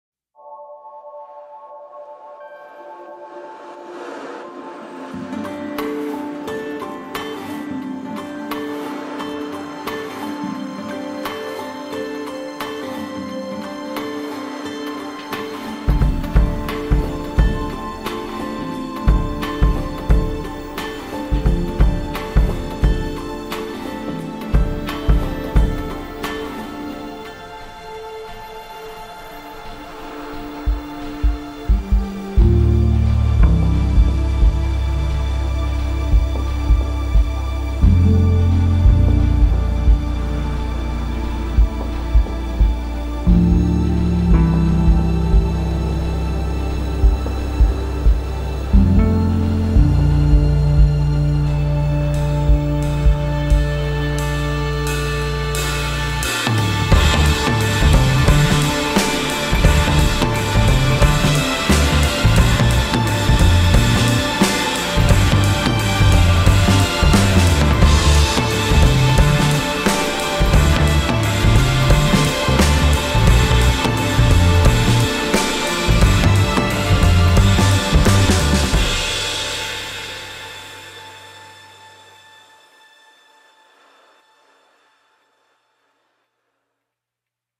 Kate Moss in Ibiza for Liu Jo Fashion Film
Behind the scenes with Kate Moss on set for Liu Jo in Ibiza shooting advertising campaign. Film made by Sam Faulkner.